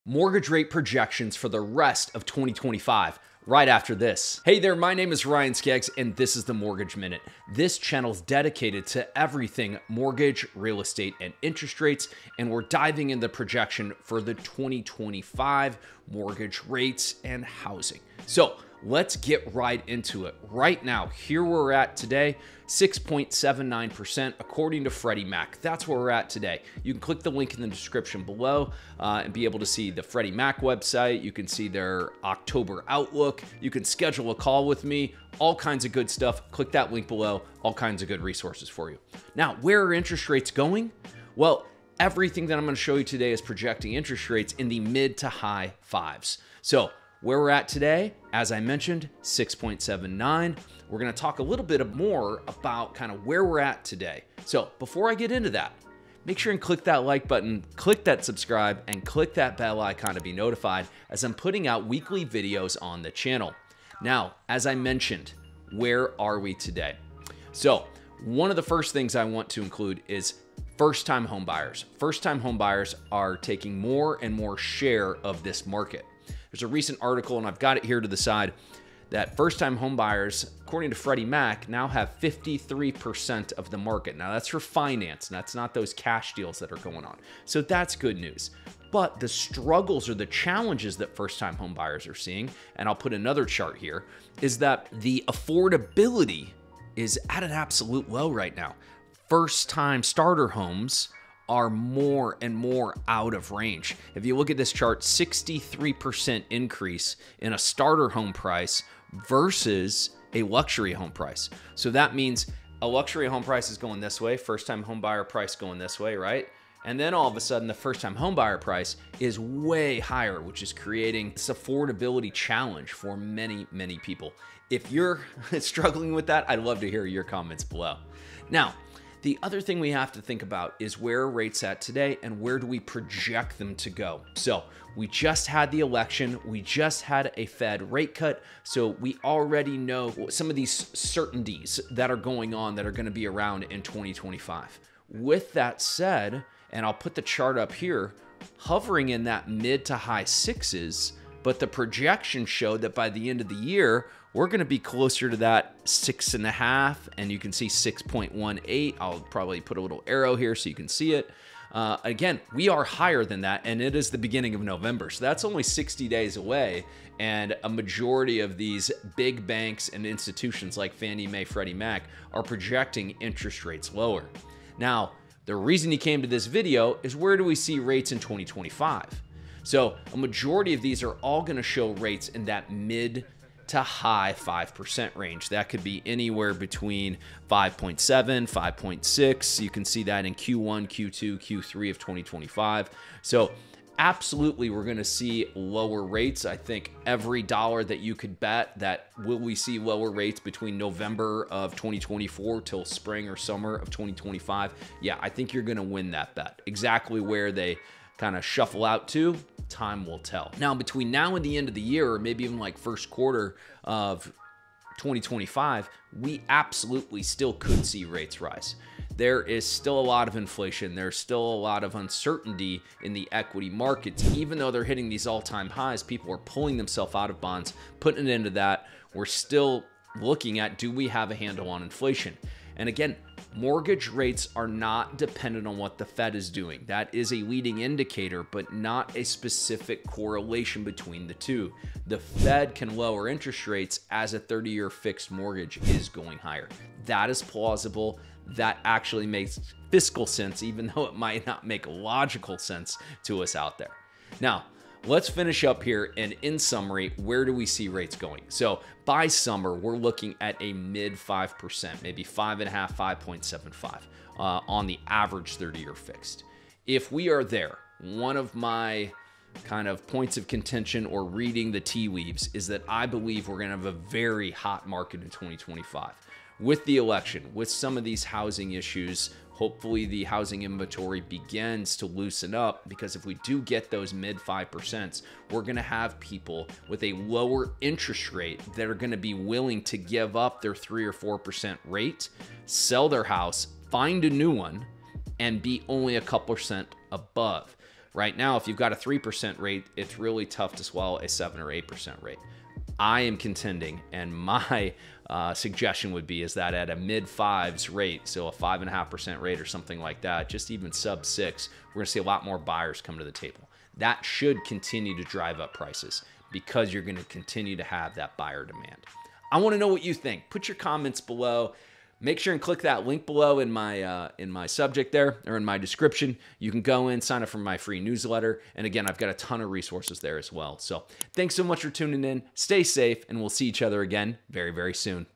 Mortgage Rate Projection | Are mortgage rates going down in 2025?
🏡 Welcome to the MORTGAGE MINUTE channel! In this video, we dive deep into the crystal ball of real estate to bring you an exclusive look at what the housing market and interest rates will hold in 2025. Join us as we analyze current trends, economic indicators, and expert opinions to provide you with valuable insights into the anticipated housing prices for the upcoming year.

📈 As we navigate the dynamic landscape of the real estate market, we'll discuss factors influencing housing prices, such as interest rates, market demand, and economic conditions.

💡 Whether you're a potential homebuyer, seller, or just curious about the future of real estate, this video is packed with information to help you make informed decisions. We'll also explore regional variations and highlight areas that may experience notable shifts in housing prices, offering you a comprehensive overview of the potential opportunities and challenges in different markets.

🚀 Don't miss out on this essential information for anyone interested in real estate!

🏆NMLS 425763 | Neighborhood Loans, Inc. NMLS 222982